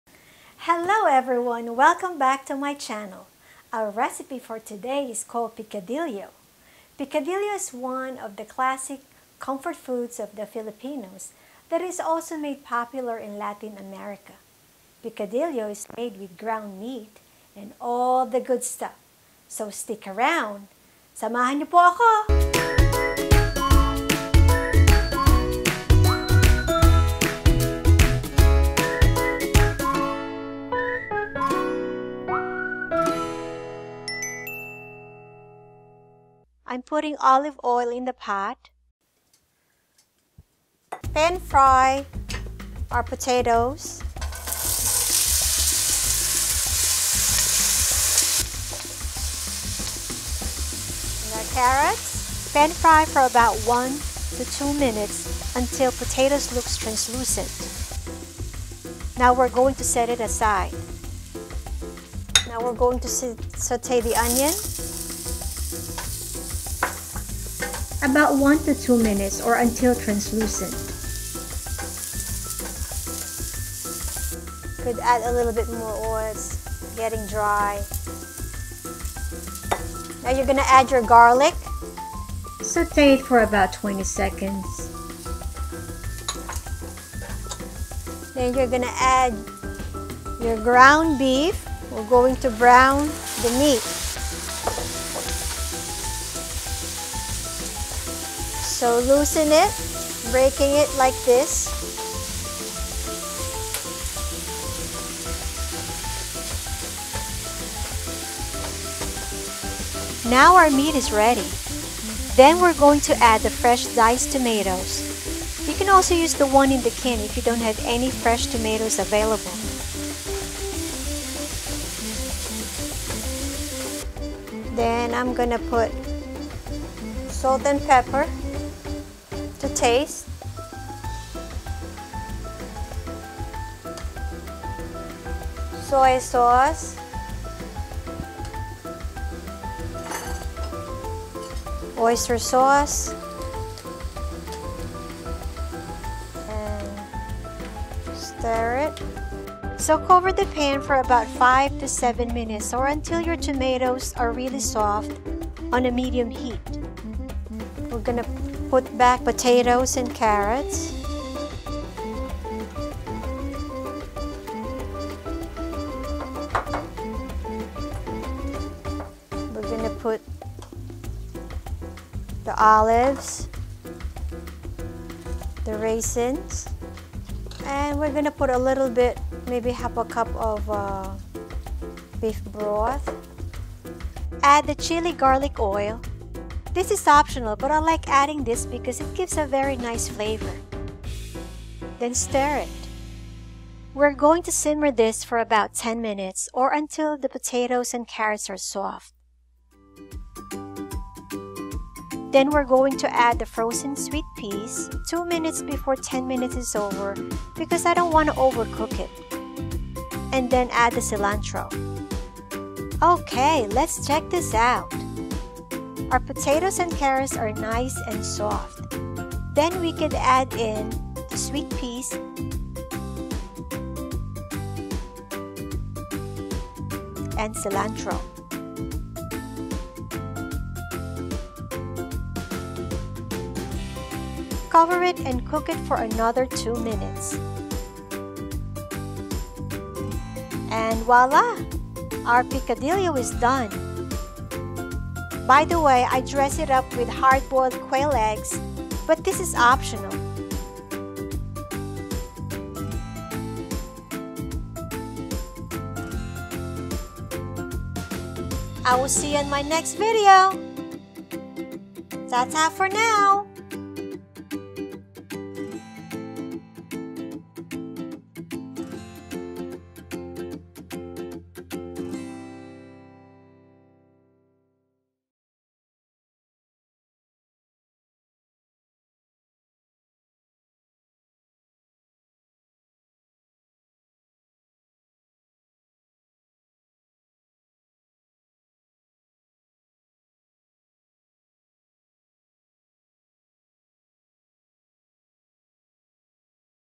Picadillo Filipino Version authentic recipe
Let us show you the easiest way to cook this ground beef recipe delight. You will learn how to make beef Picadillo the Filipino version. Join us on this journey and keep variety in your life.

The Picadillo recipe:

2 lbs ground beef
1 small onion chopped
5 cloves garlic chopped
2 carrots – diced or cubed
3 medium size potatoes – cubed
1 cup frozen sweet peas
4 medium size fresh tomatoes – diced or 1 small can tomato sauce
1 dozen quail eggs hard boiled – optional
1oz raisins
¼ cup green olives
1 tbsp oyster sauce
1 tbsp soy sauce
salt and pepper to taste
½ cup beef broth or water
1 tsp garlic
chili oil to taste – optional
Cilantro - chopped

Picadillo is a traditional dish in Latin American countries including the Philippines. It is made with ground meat, tomatoes, raisins, and green olives. Other ingredients vary by region.  It is often served with rice.

CREDITS:

BACKGROUND:
Wood Photo by Keith Misner on Unsplash

OPEN:
Mukbang Food Youtube Intro - Regular License

TRANSITIONS - LOWER THIRDS - CALL OUTS - BEGIN & END SLATES
Youtube Pack - Transitions - Regular License